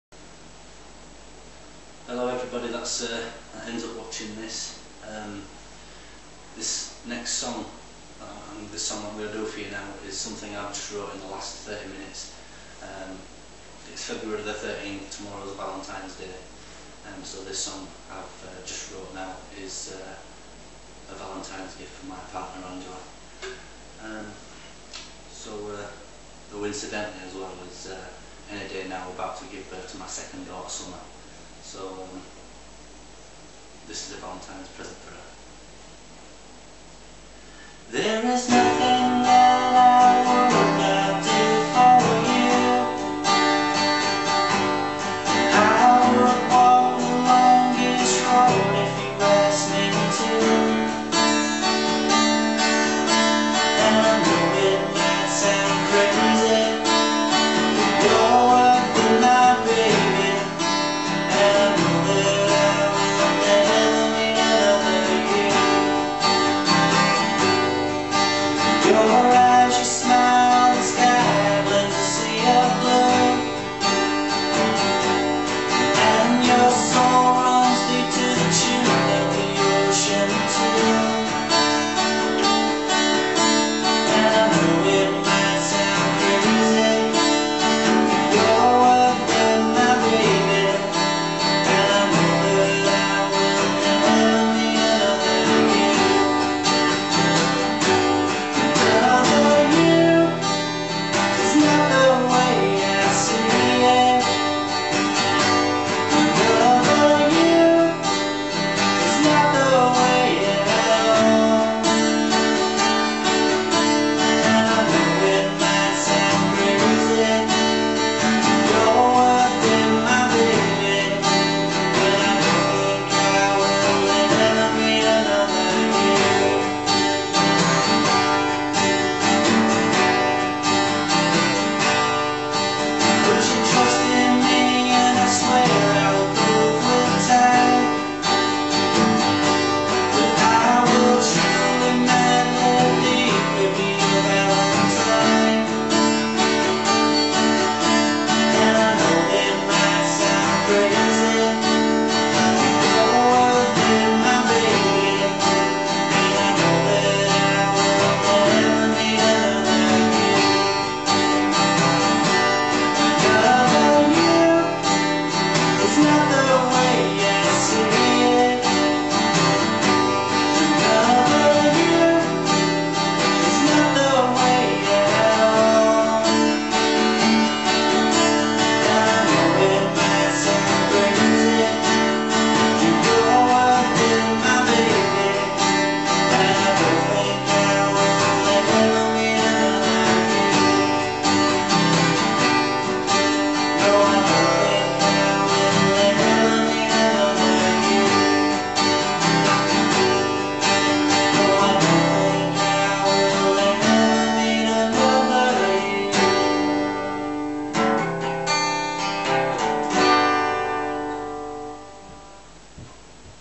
Valentines gift! (original)
This is a song for my partner Angela. I wrote it in 30 minutes, and is a suprise Valentines day present for her.